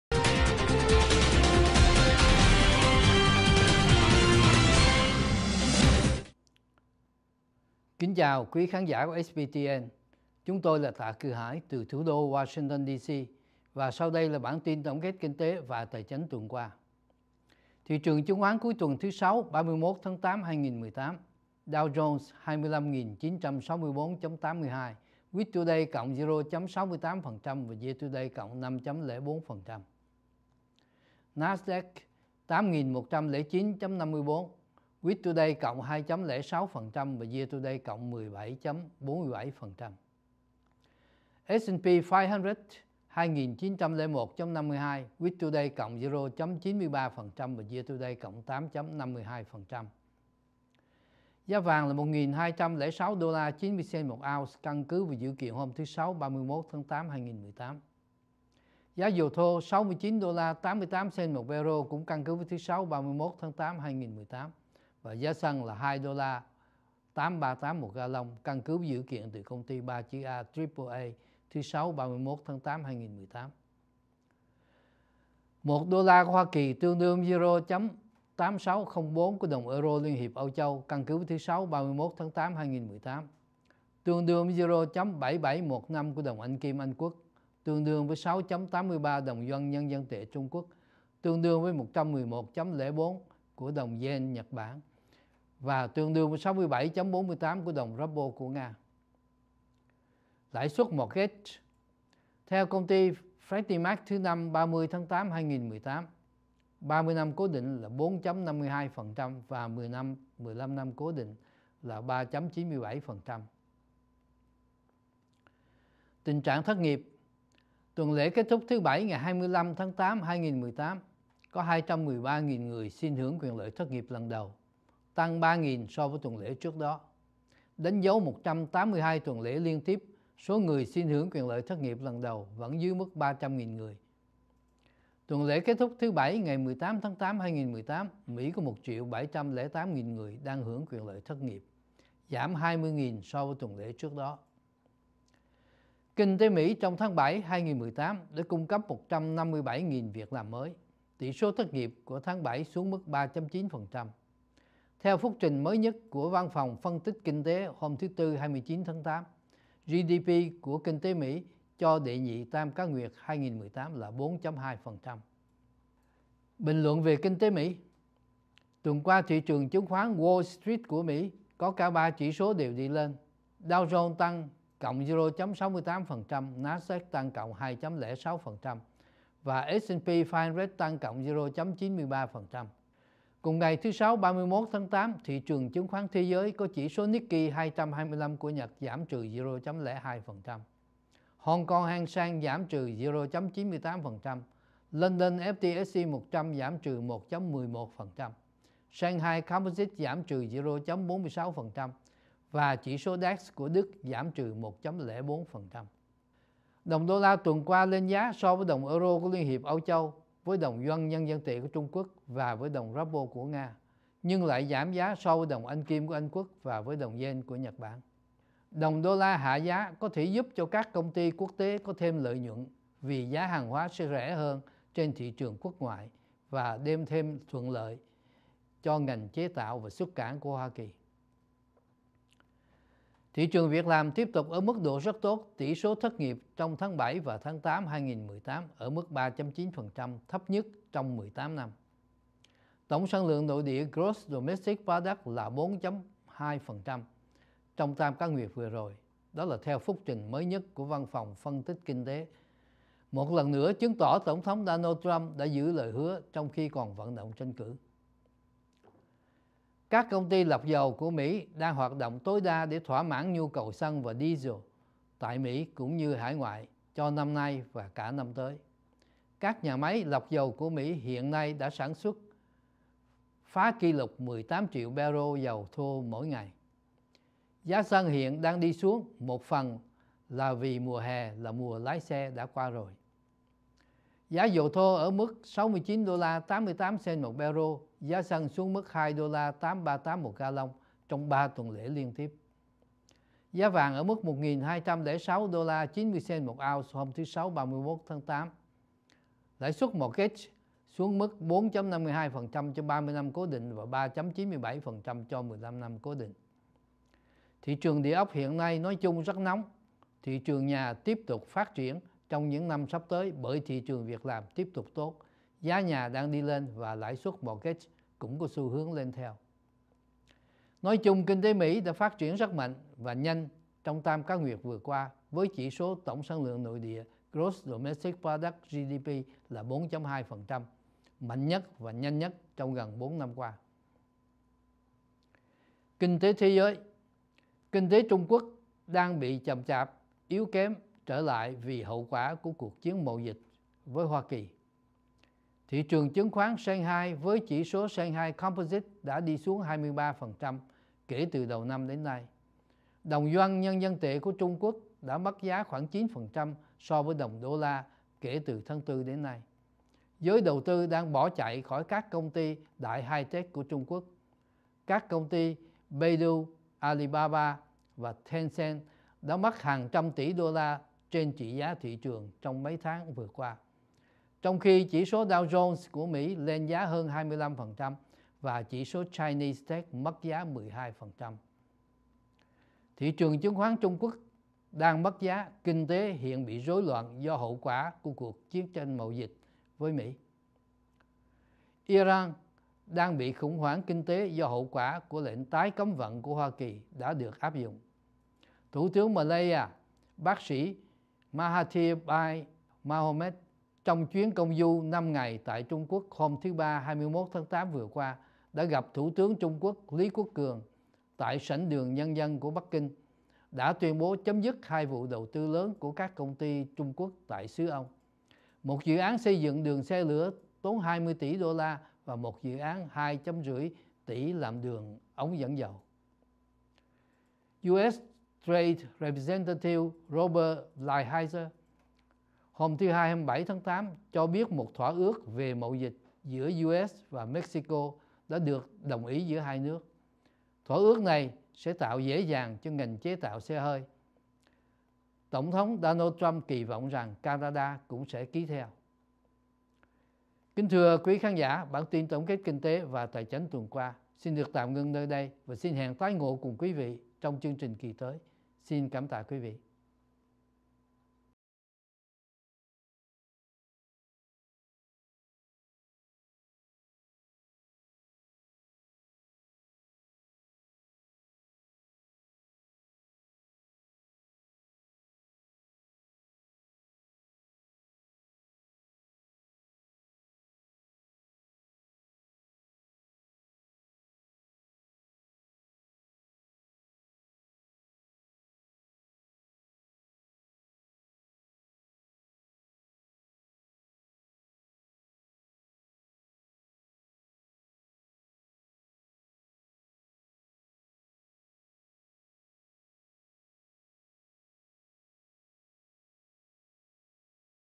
VATVOnline News: Tổng Kết Kinh Tế và Tài Chánh Tuần Qua Với Tiến Sĩ Tạ Cự Hải
02/09/2018